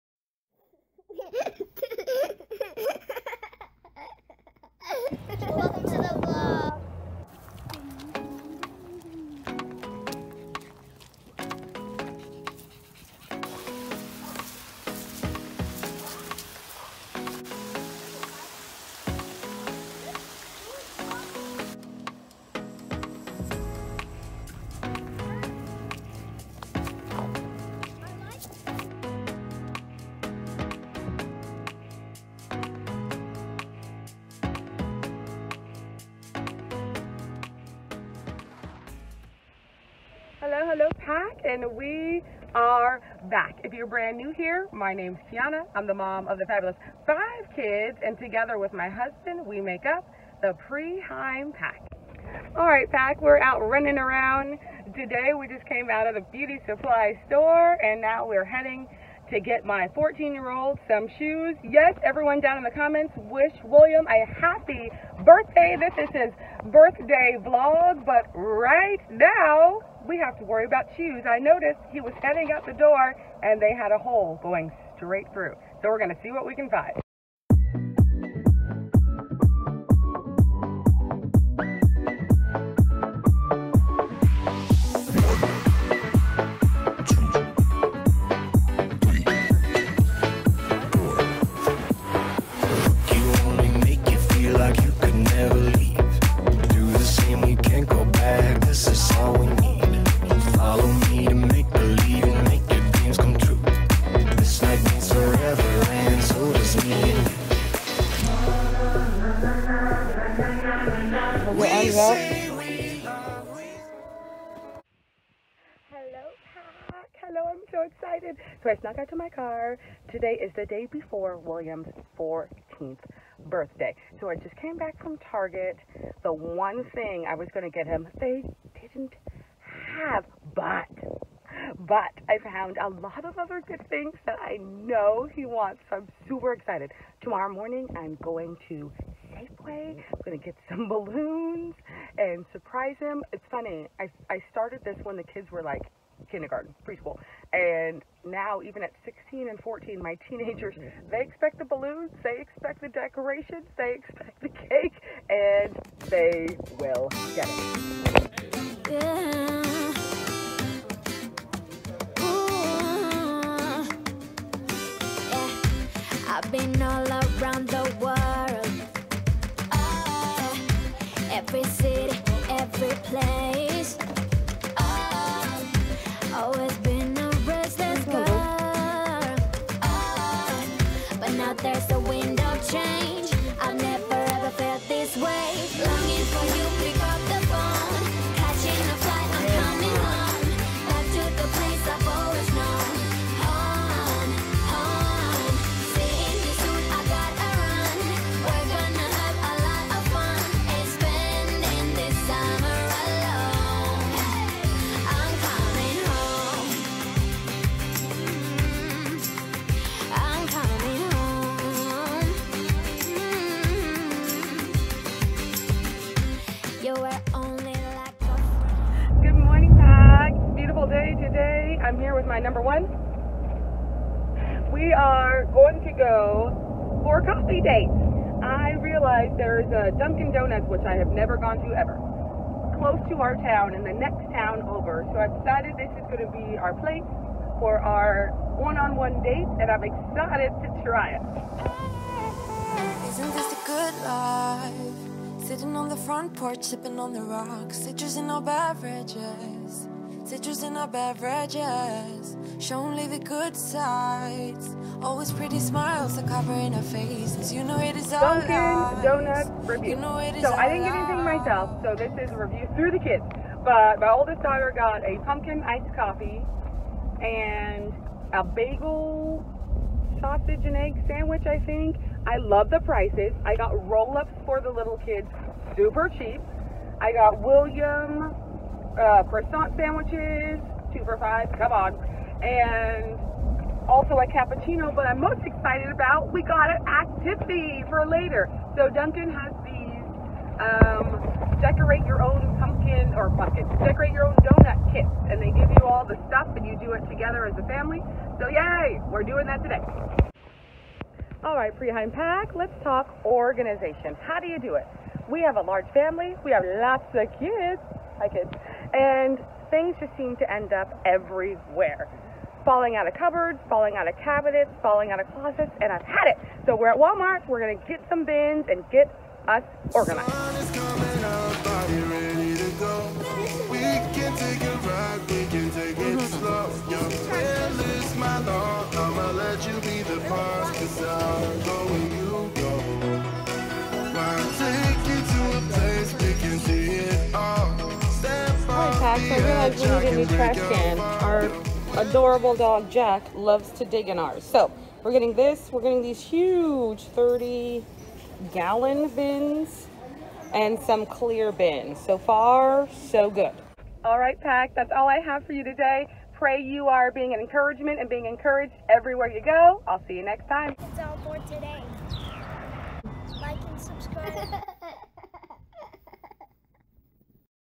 Making Everyone in a Large Family Feel Special
We are busier than ever but it's so important to constantly celebrate every member of our family!